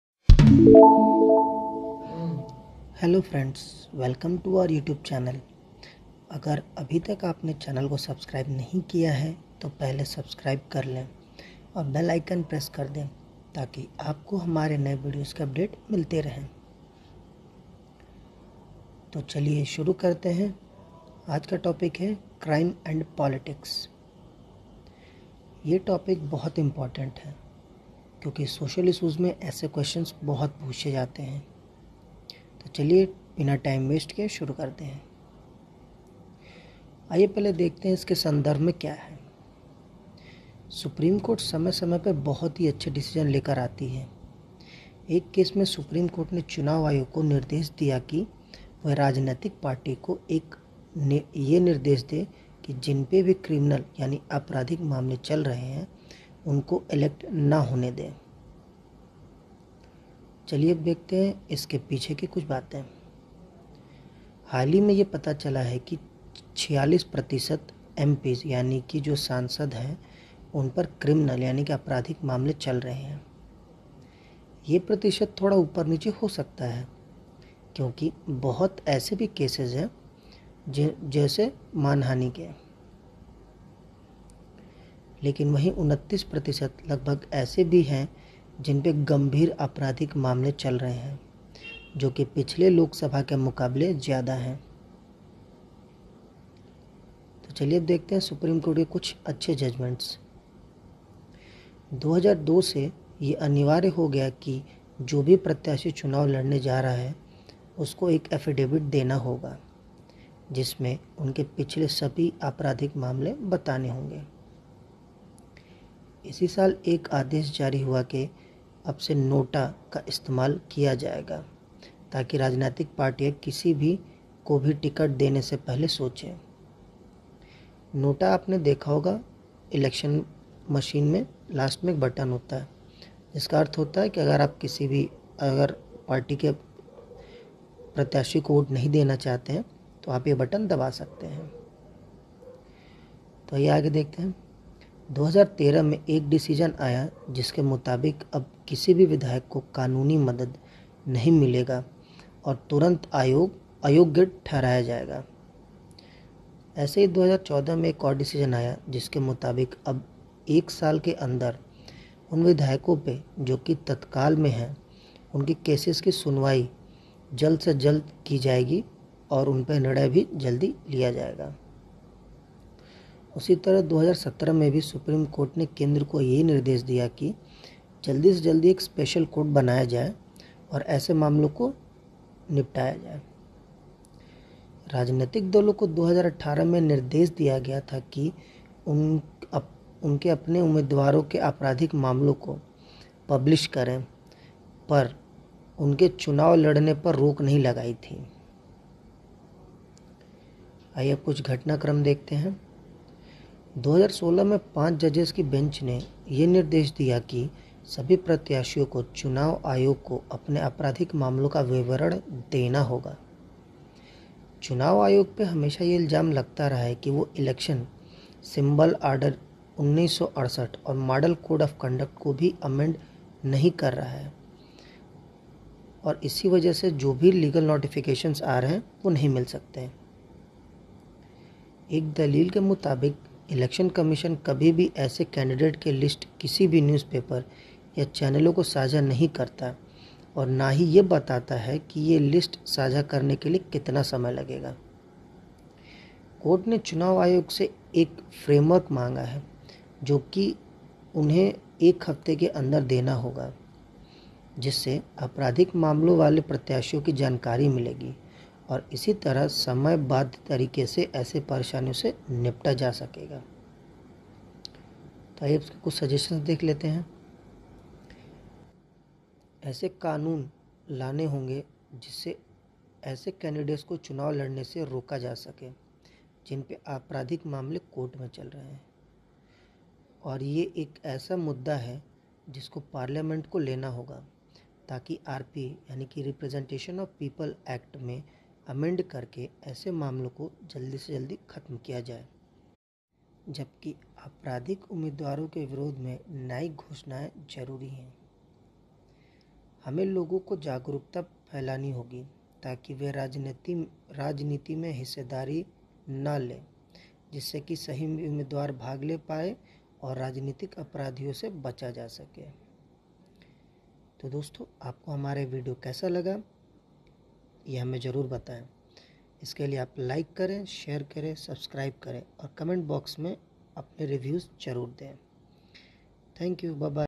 Crime and Politics
Salient Features of Representation of People's Act (RPA).
Criminalisation of Politics.
Supreme Court Judgements.
Ways to Decriminalize Politics.